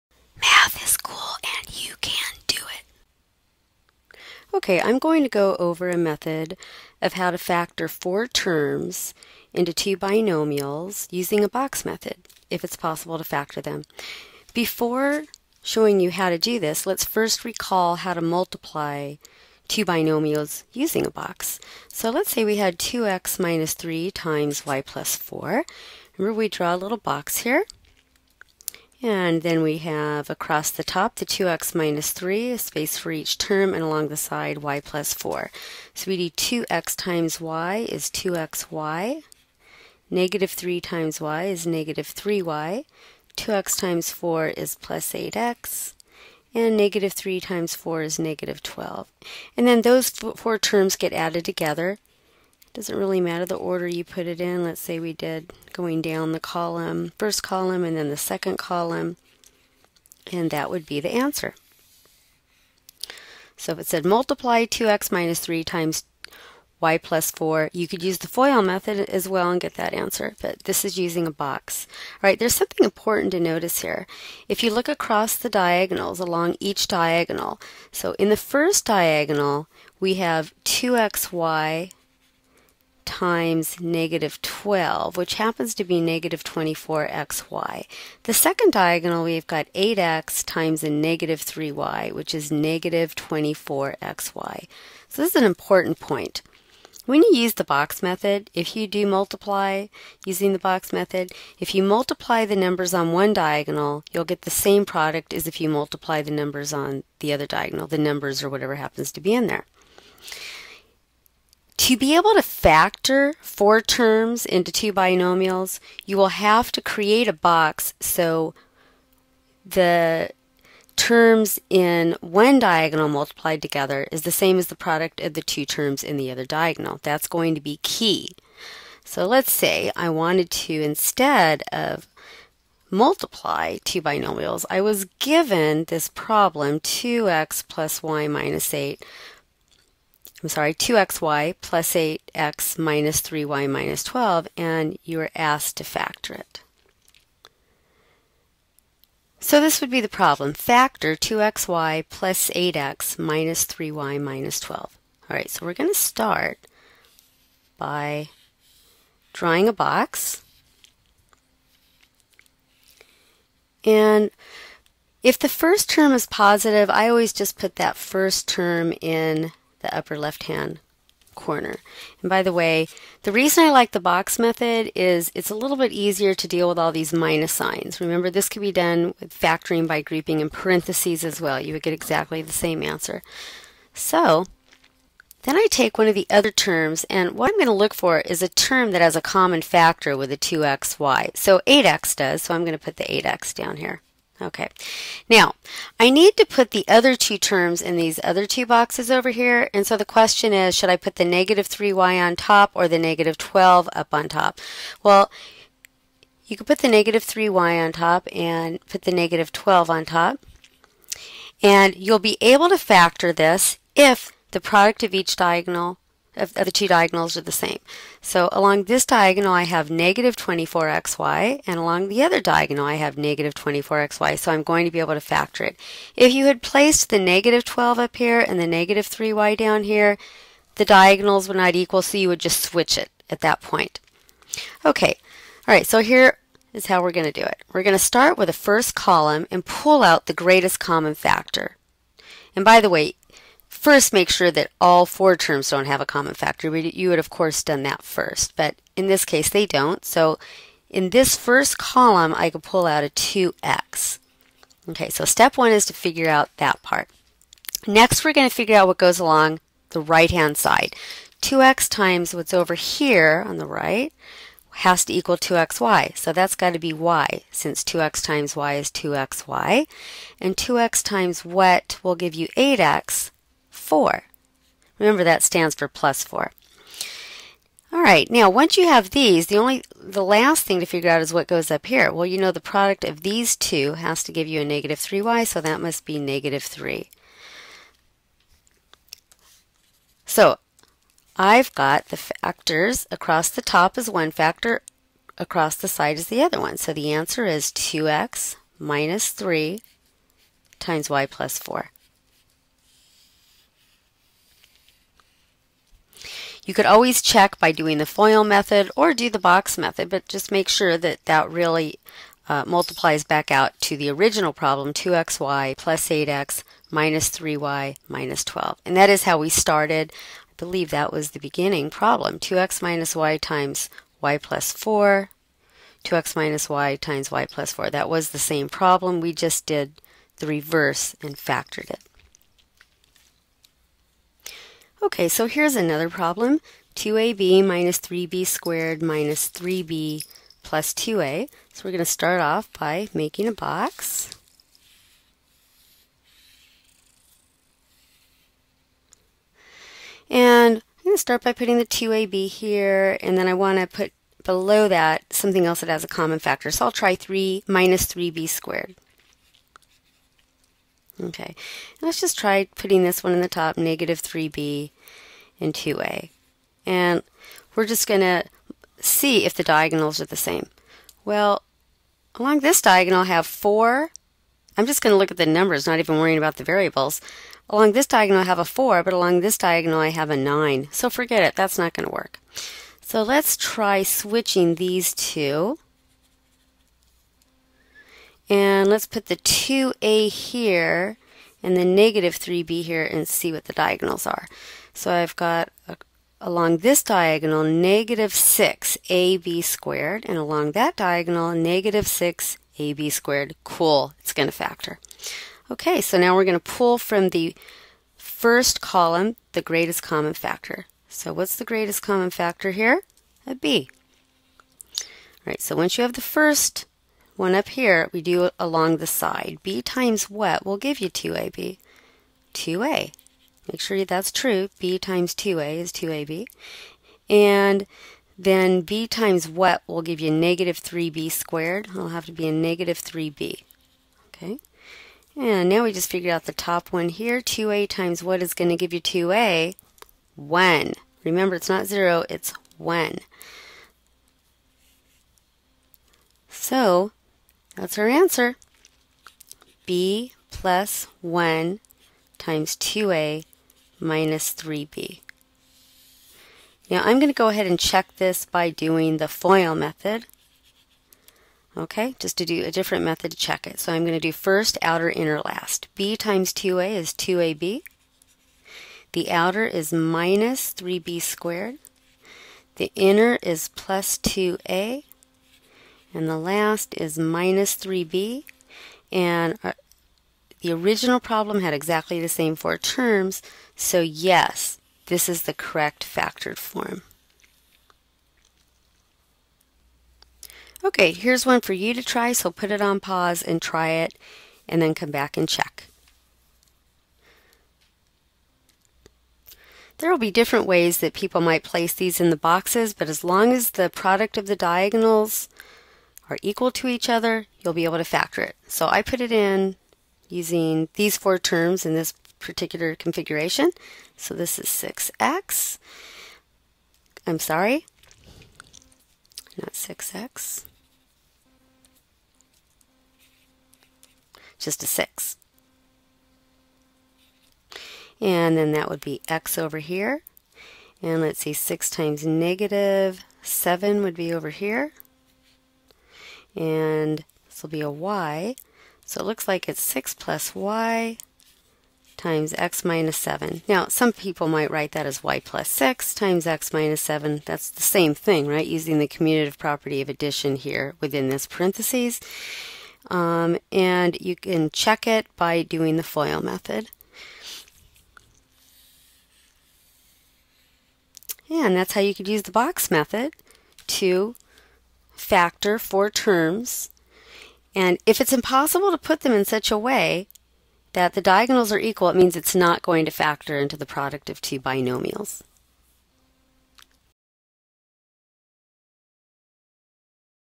Factoring 4b-Use Box to factor 4 terms
This video covers how to factor 4 terms into 2 binomials by using a nontraditional method using a BOX.